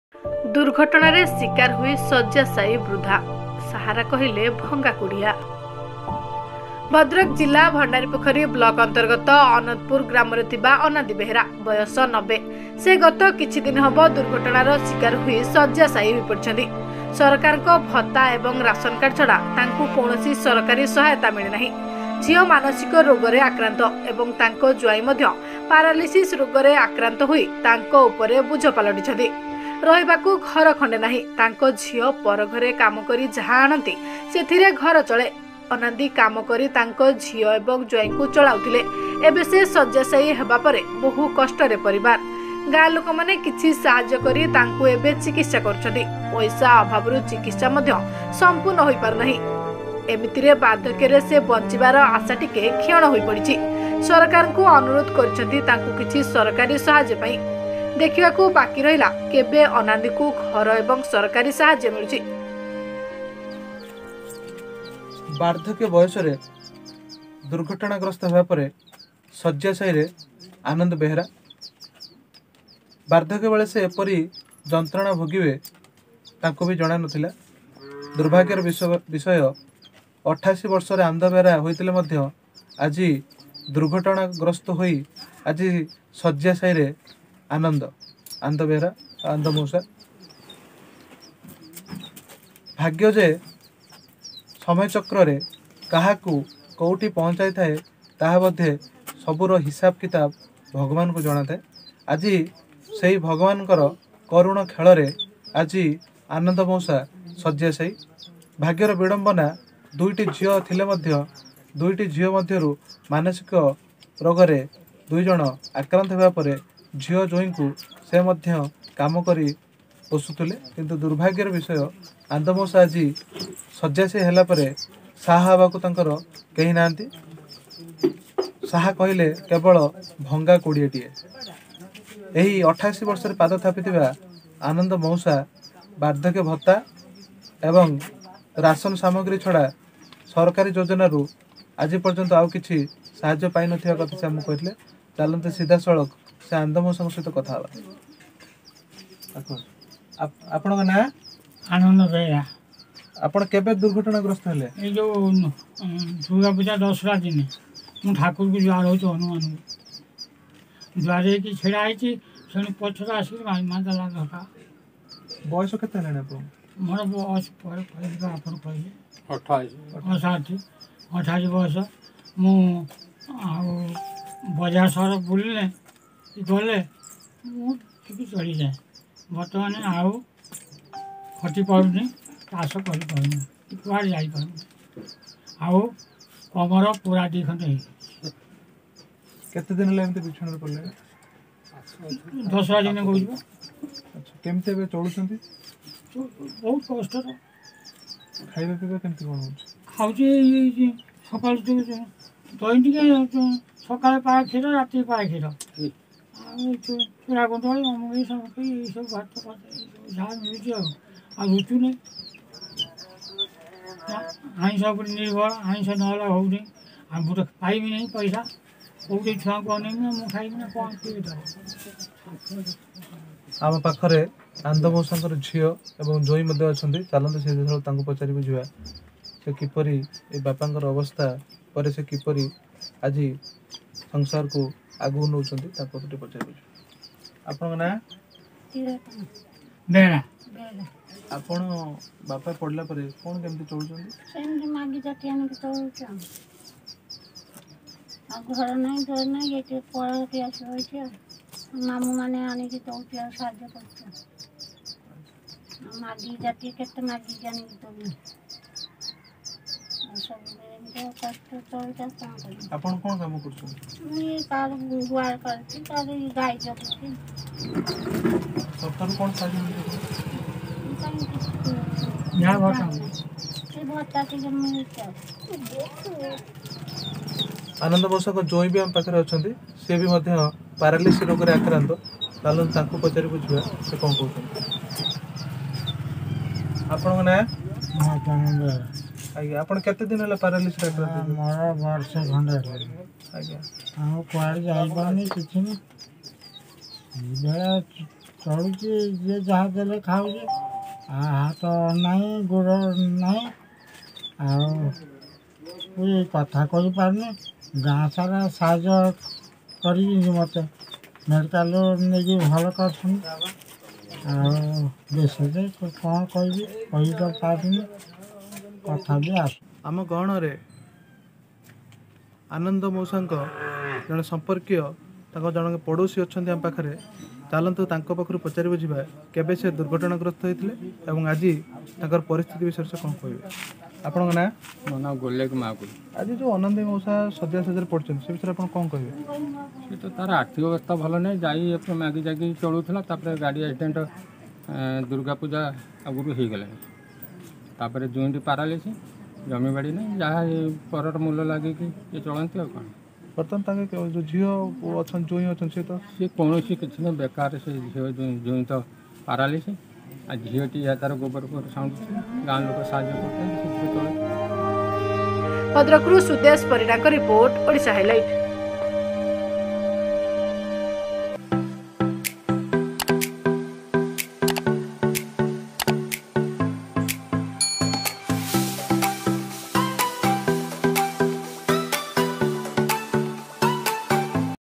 ଦୁର୍ଘଟଣା ରେ ଶିକାର ହୋଇ ଶଯ୍ୟାଶାୟୀ ବୃଦ୍ଧା, ଦୁଃଖ ଦୁର୍ଦ୍ଦଶା ରେ ପରିବାର ଲୋକେ || Odisha Highlight News
Odisha Highlight News

more updates 👉